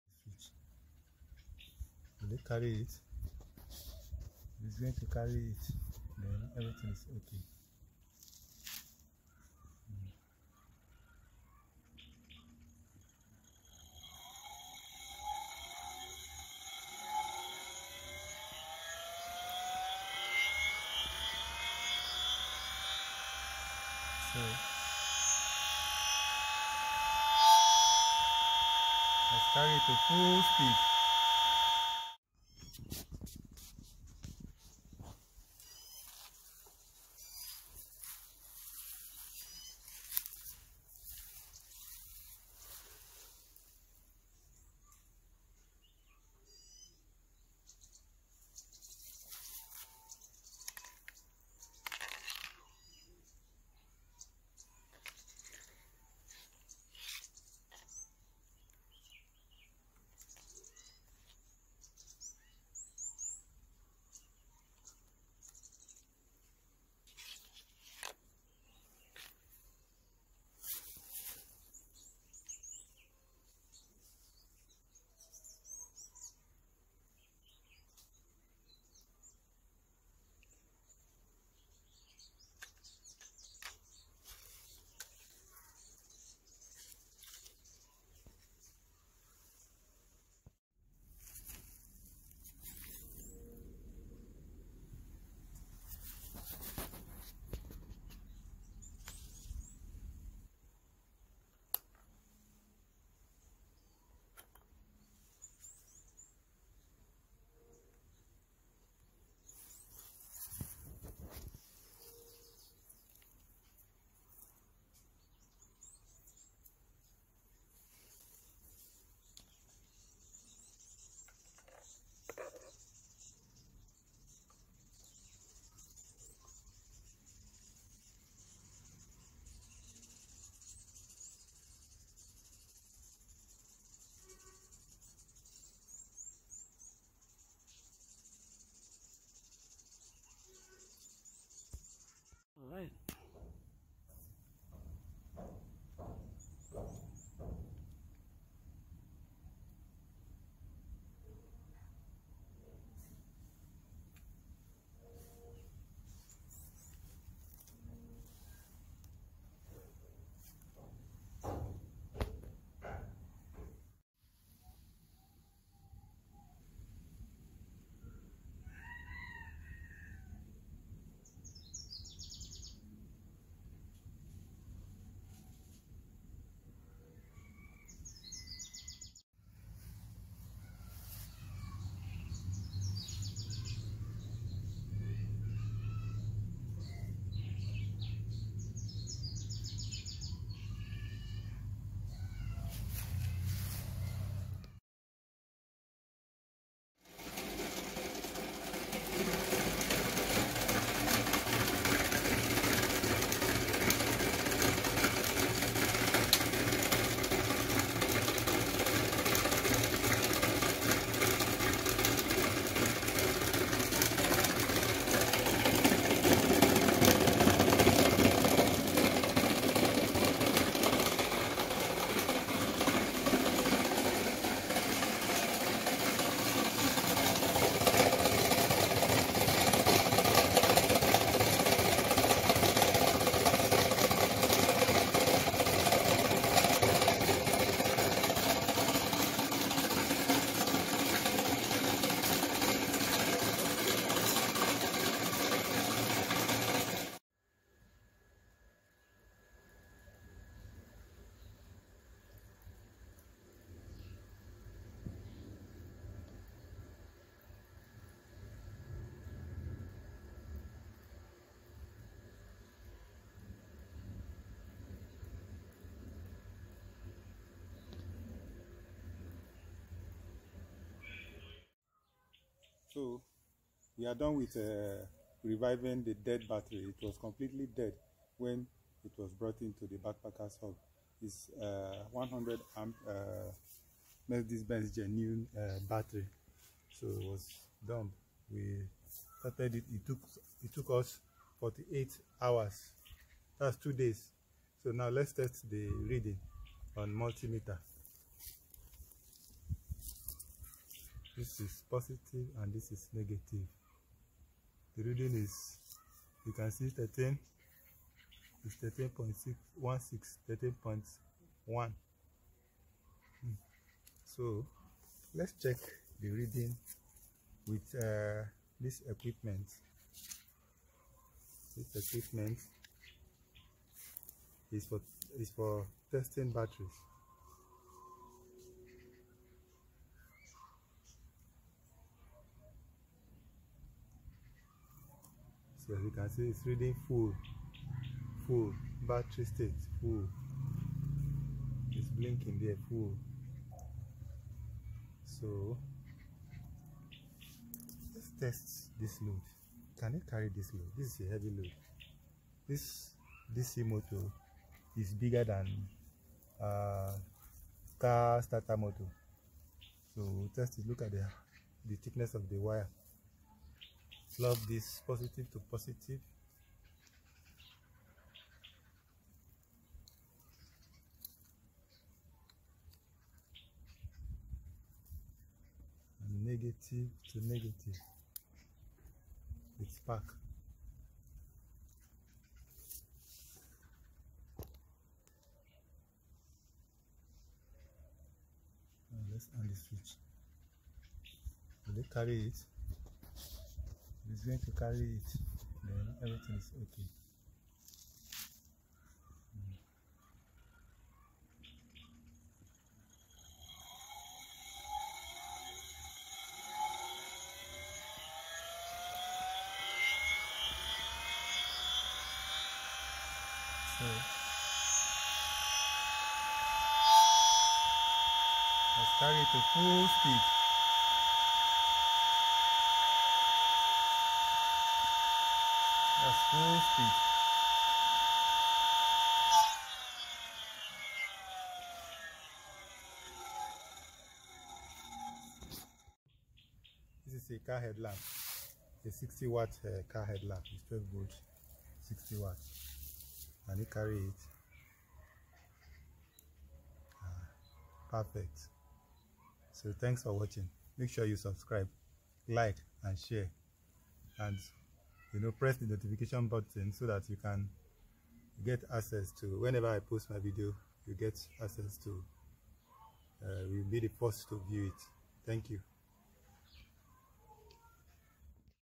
How to bring Dead Battery back to life, Pulse Motorgen, Free Energy
In this video I show the processes involved to bringing back a lead acid car battery to work again as if it were new.
A follower brought in the dead battery from his Mercedes Benz after several failed attempts to charge it, he could not use the car as a result. he watched my last video on Pulse motorGen and gave me a visit with the battery.
He can now drive his luxury car around thanks to RobertAdams/Bedini's work: My modified Pulse motor generator.
To get virtual paid assistance to construct one, and/or revive your dead batteries...
thanks.